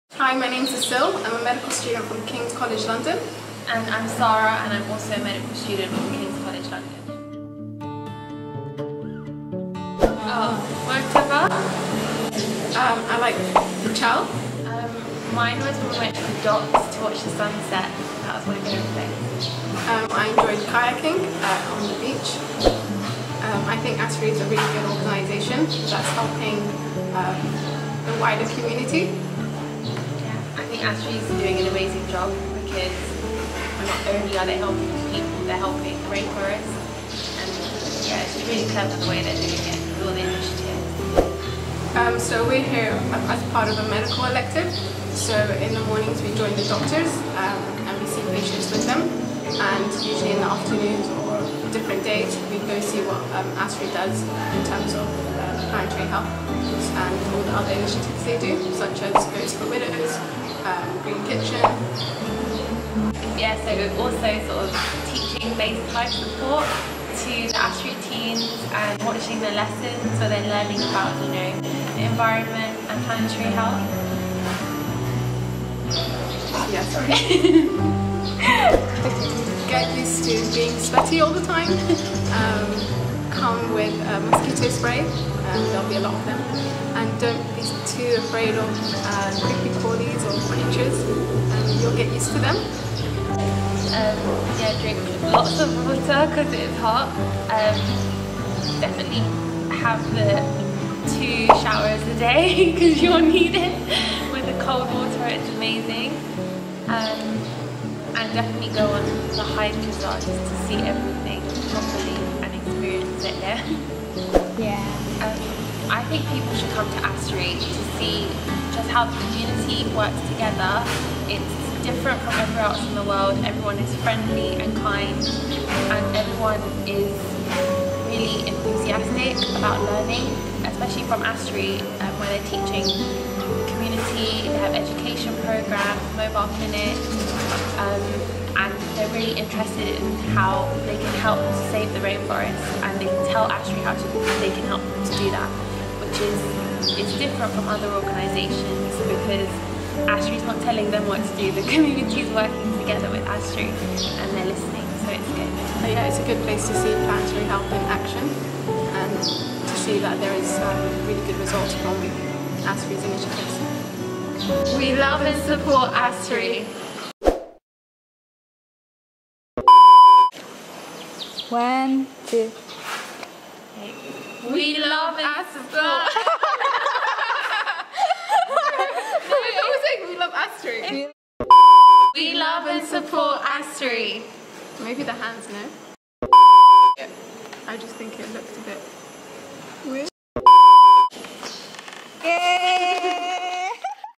QnA With Asil and Sarah: Borneo Rainforest Experience in Sukadana
Asil and Sarah are ASRI visitor. They came to Sukadana to see Planetary Health in Action at ASRI and discover the beauty of Borneo rainforest.